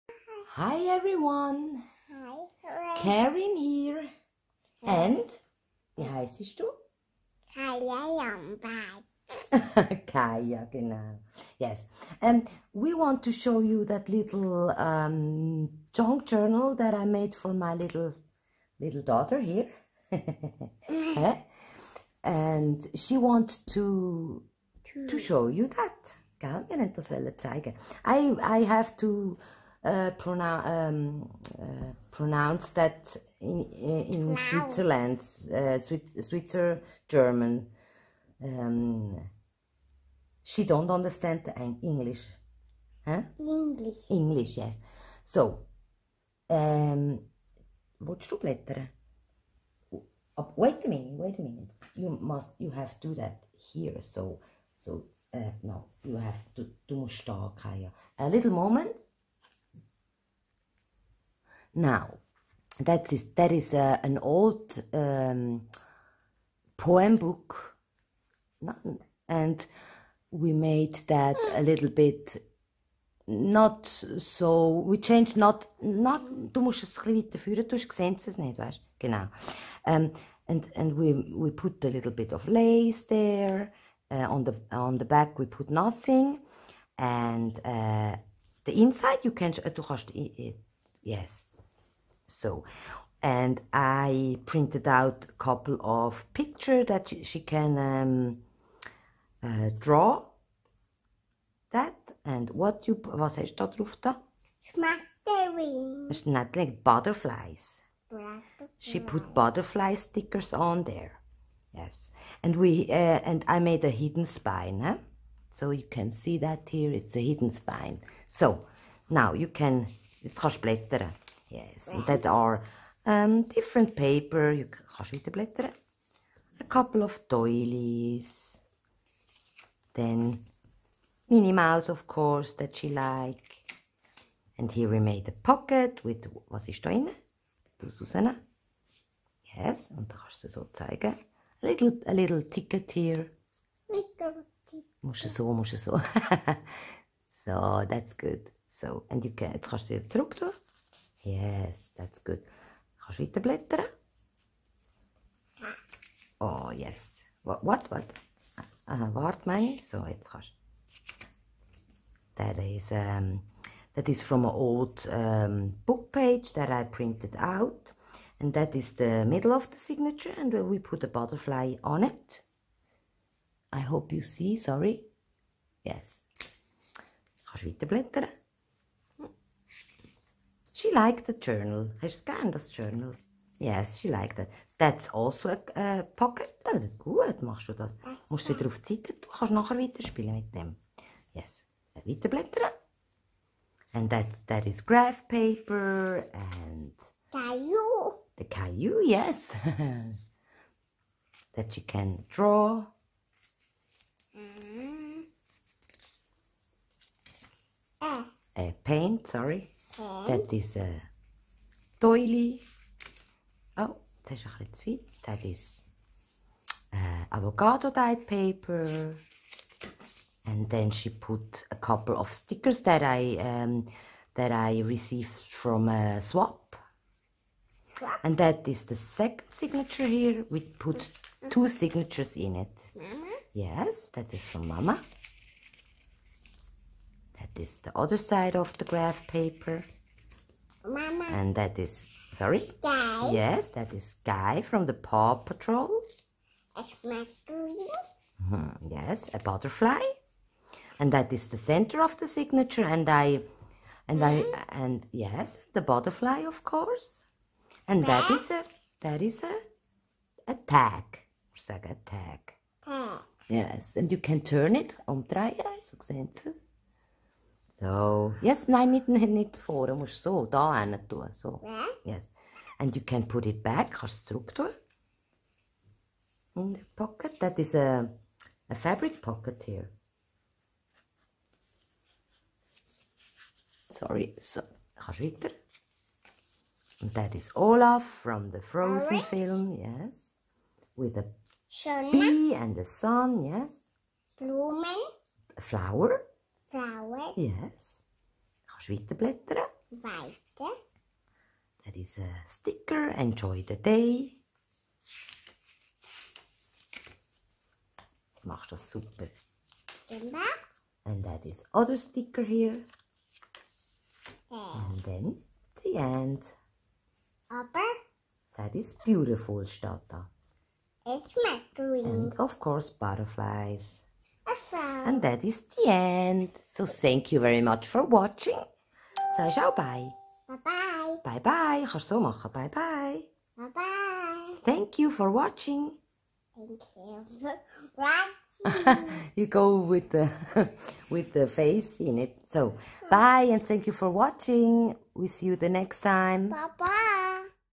Flip trough mini junk journal for my daughter, with my special guest :)
Hi everyone, i made that little junk journal for my daughter Kaya and i want to share that with you. I had a little helper in the video.